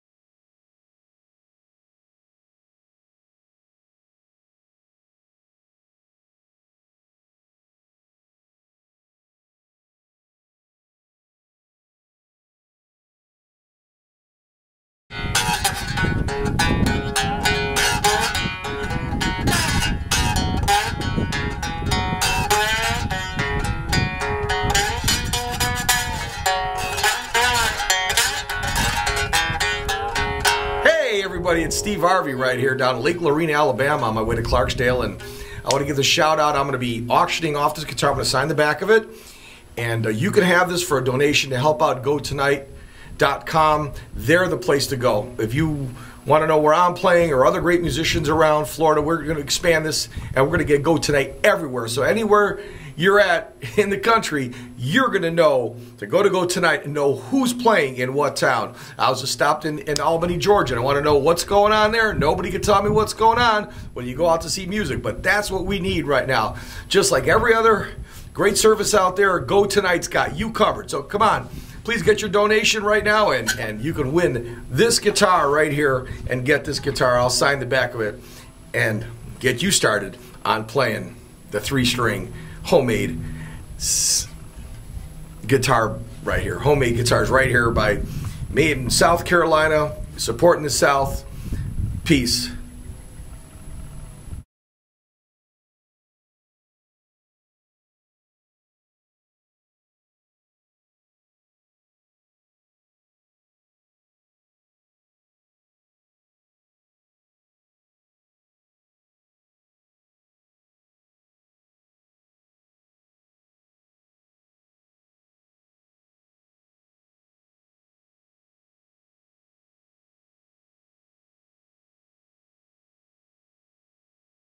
GoTonight GoFundMe
A thoughtful and generous infomercial produced by Steve Arvey for the upcoming GoFundMe campaign for GoTonight.com, Helping you find local live music!  Thank you Steve!